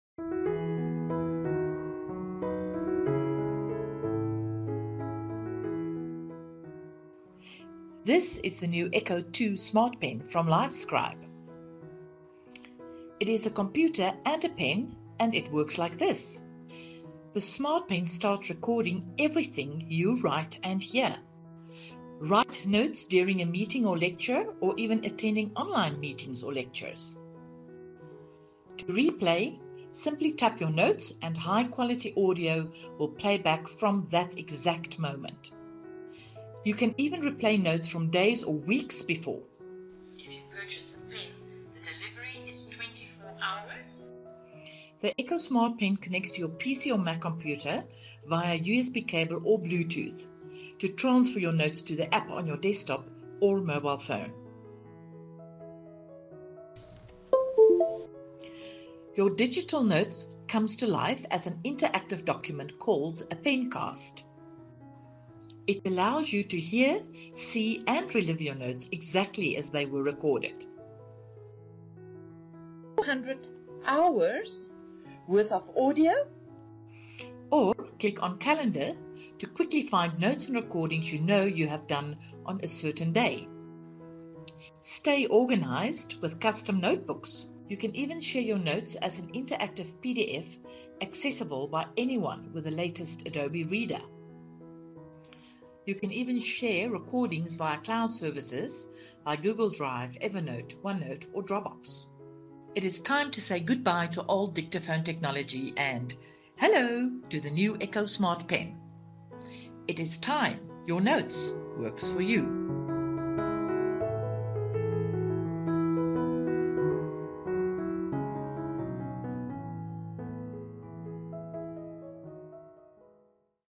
A pen that remembers who said what??
This is my latest project that I have done - marketing video for Cost Recovery Solutions.

Fun facts about the pen:
• Memory capacity: 4GB (200 hours audio) and 20 000 A4 pages written text storage
• Audio recording with notes
• Built-in Microphone and Speaker on pen
• 8.5 hours continuous writing time
• Convert handwriting to text
• Supported Platforms: Android, iOS, Windows, MacOS
• Real time streaming
• Auto Sync notes with Cloud Services: Google Drive, Dropbox, Evernote, OneNote
• Connectivity: Wired USB and Bluetooth